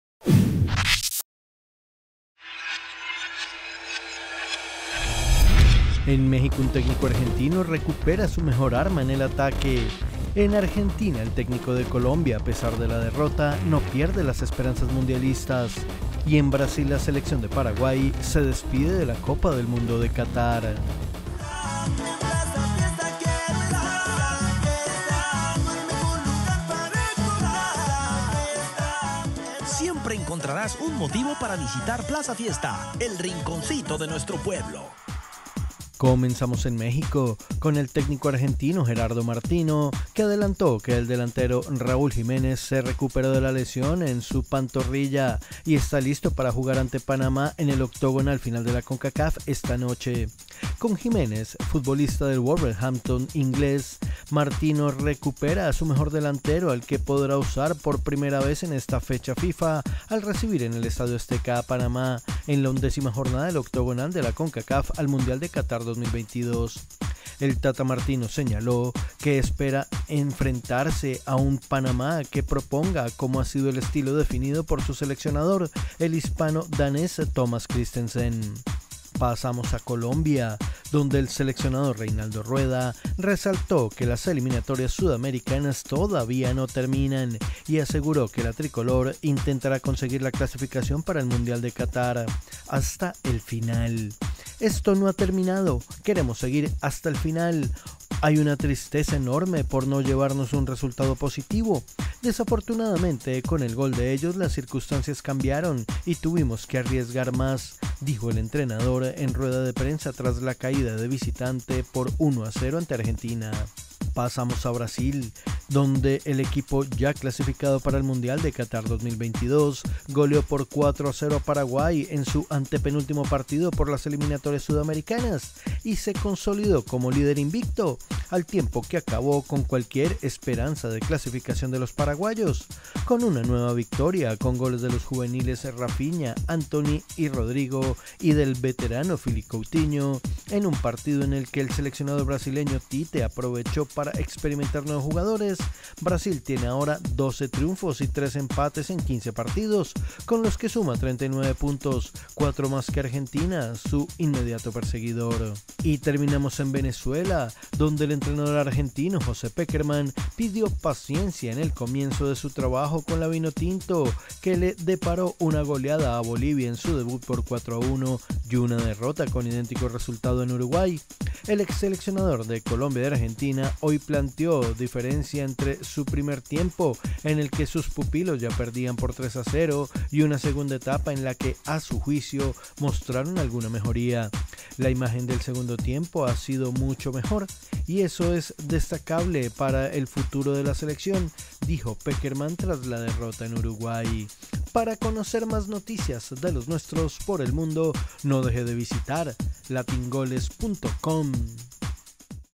En Colombia su técnico aun cree que puede clasificar al mundial
En México un técnico argentino recupera a su mejor arma en el ataque, en Argentina el técnico de Colombia de la derrota no pierde las esperanzas mundialistas y en Brasil la selección de Paraguay se despide de la Copa del Mundo de Qatar